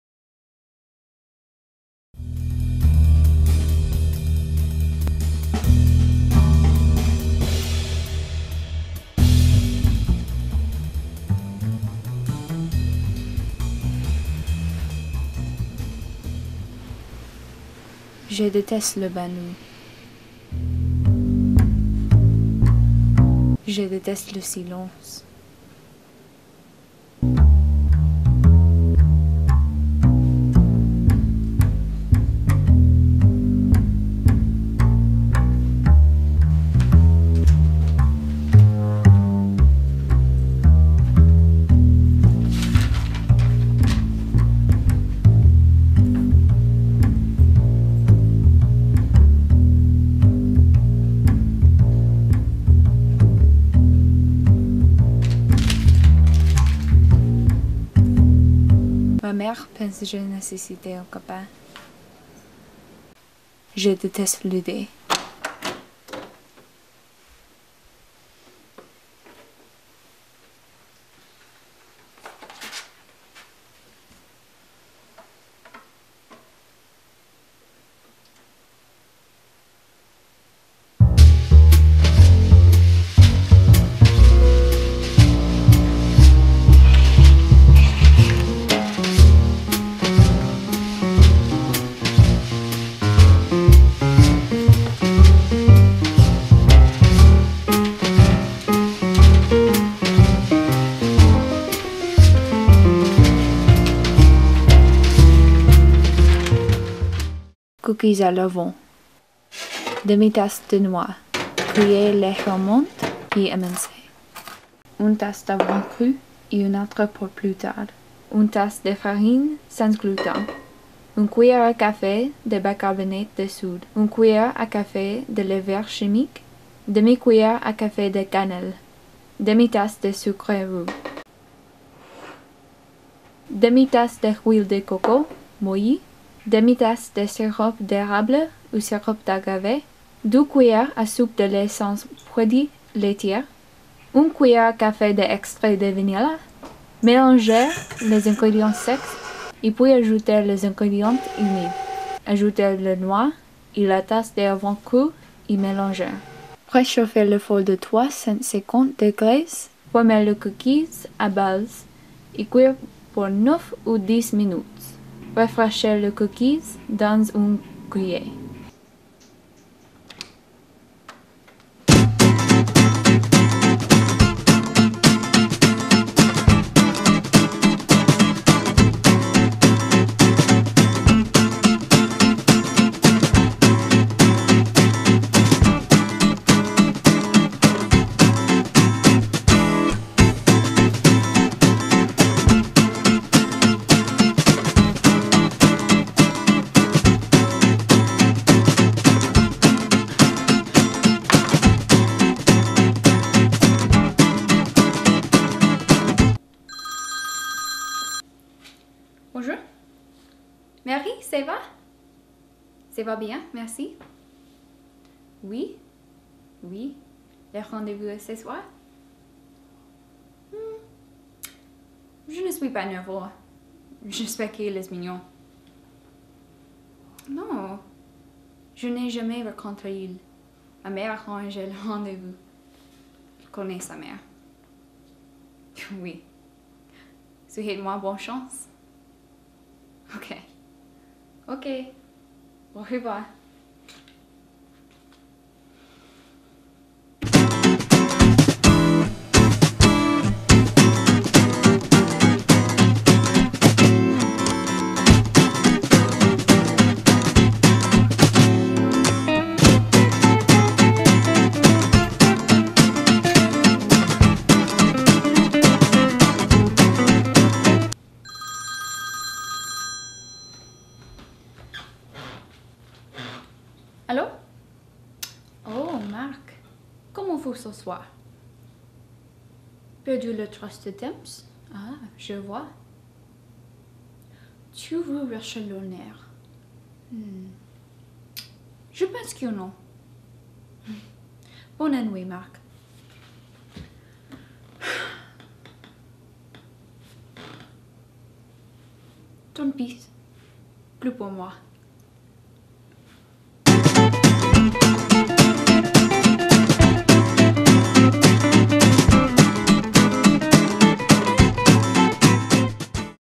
The Jean-Luc Godard Cooking Show
Learn how to make vegan and gluten free oatmeal cookies a la Jean-Luc Godard, the legendary French director of the 60's!